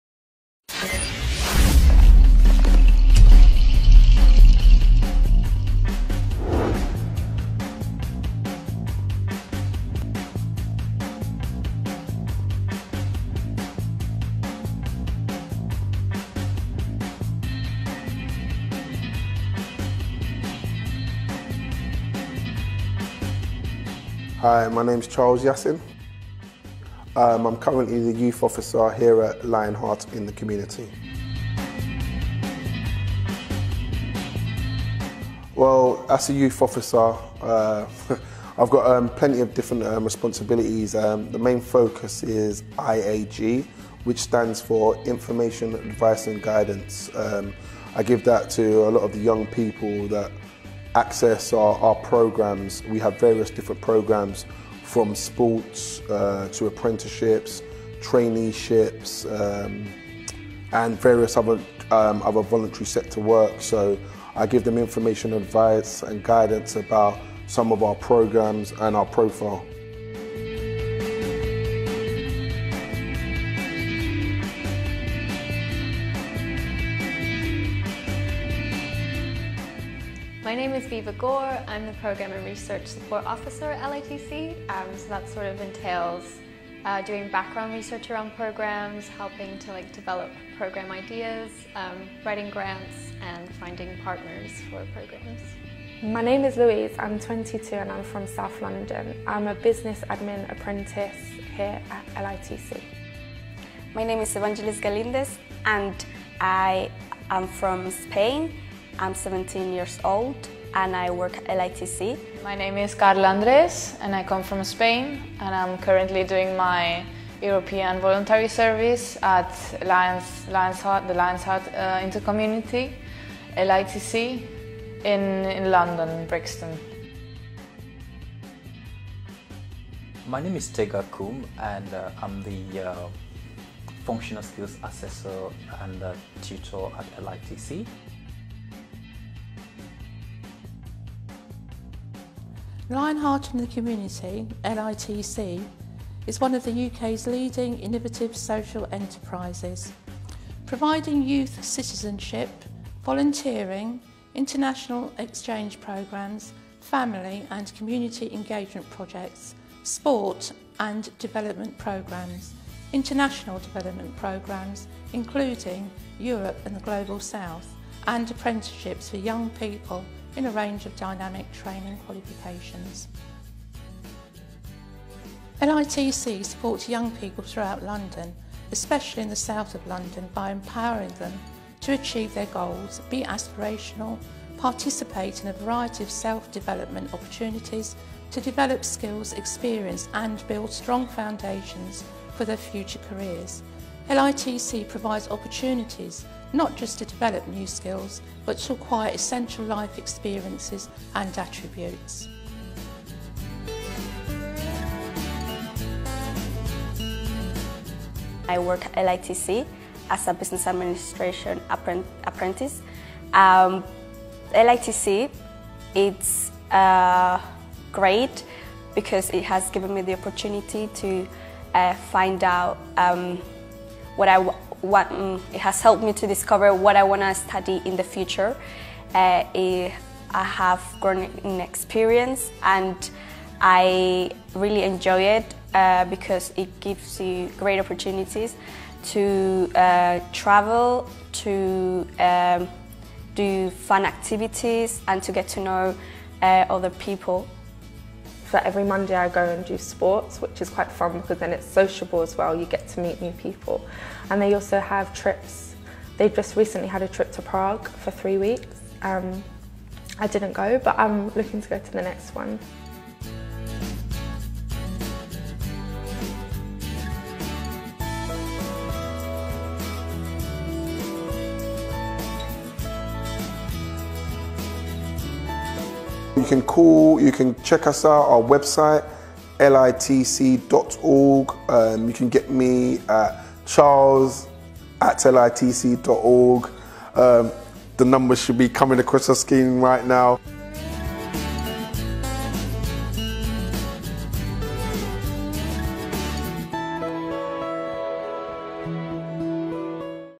LITC an Introduction
Empowering Local Communities through Sport
LITC is a pioneering NGO mobilising the power of Sport to empower our local Communities. LITC offers International Youth projects, Volunteering, Skills development and Apprenticeships in a range of dynamic training qualifications.